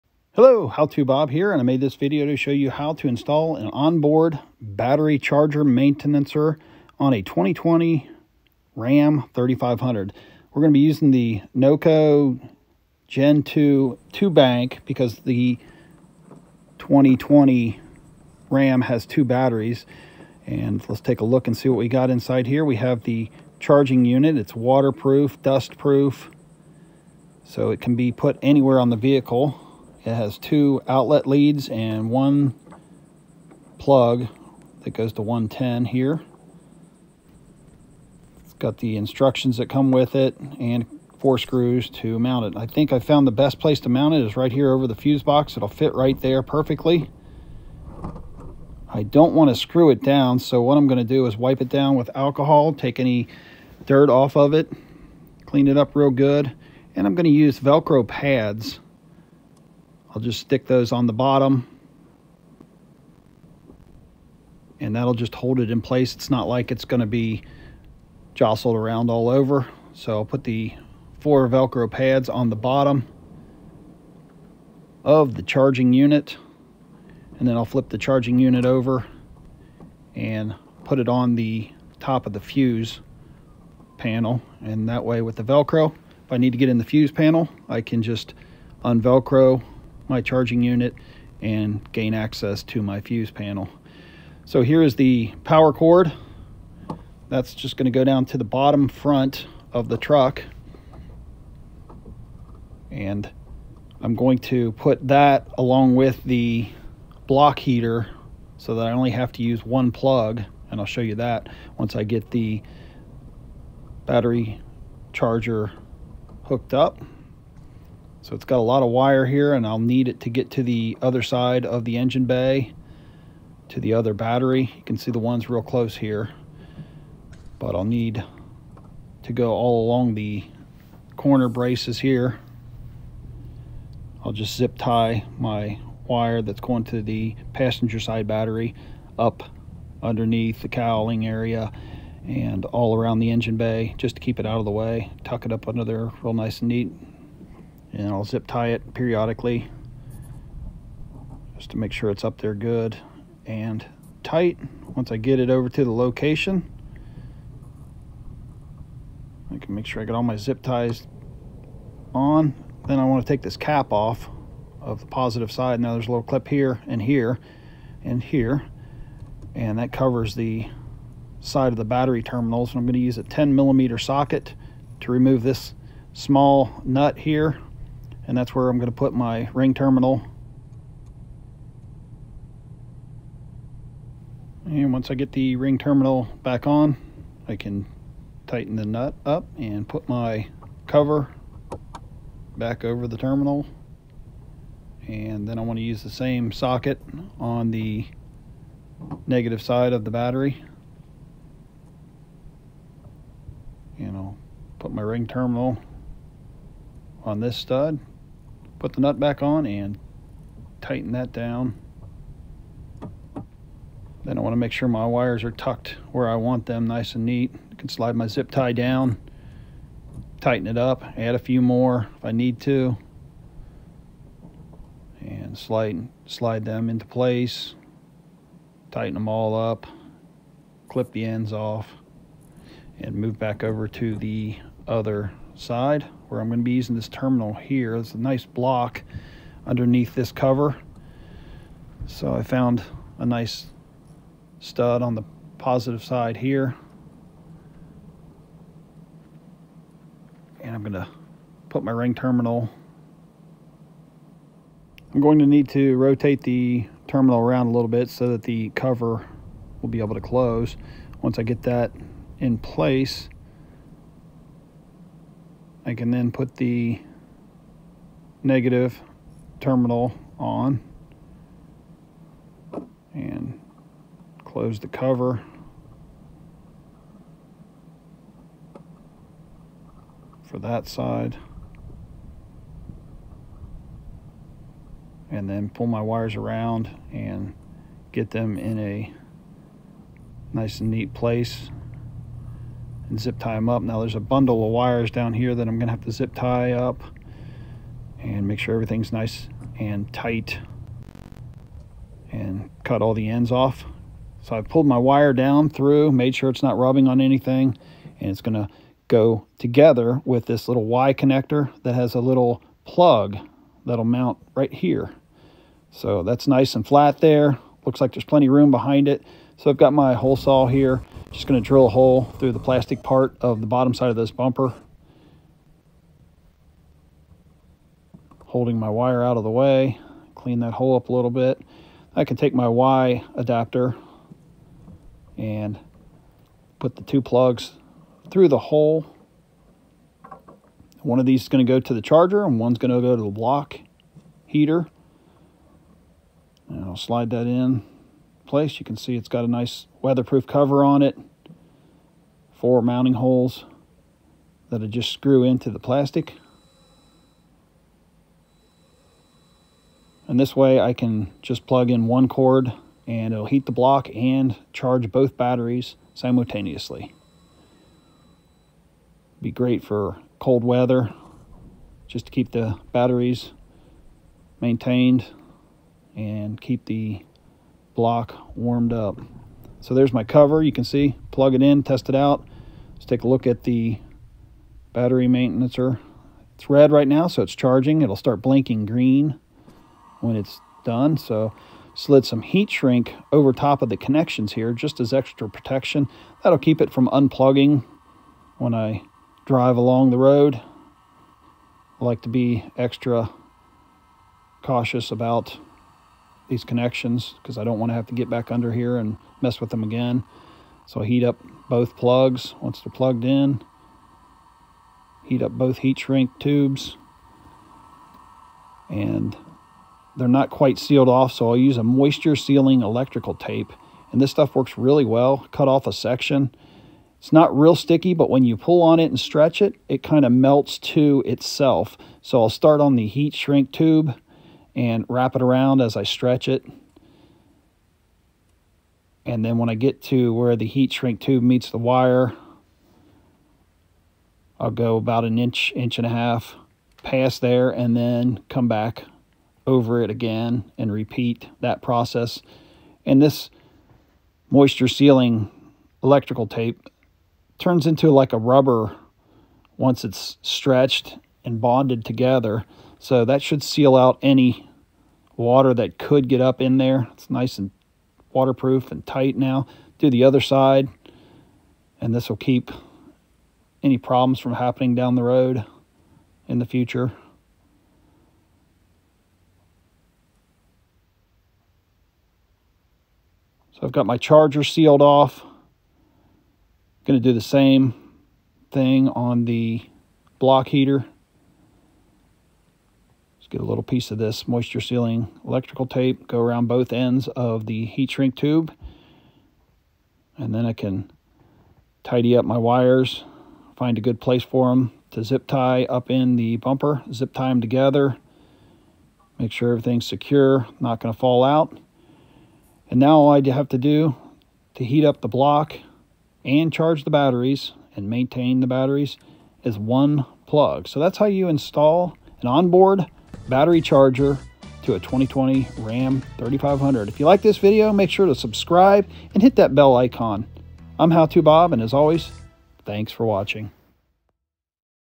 Installing an On-Board Battery Charger
In this video I will install a Noco On-Board battery charger/maintainer to a 2020 Ram 3500.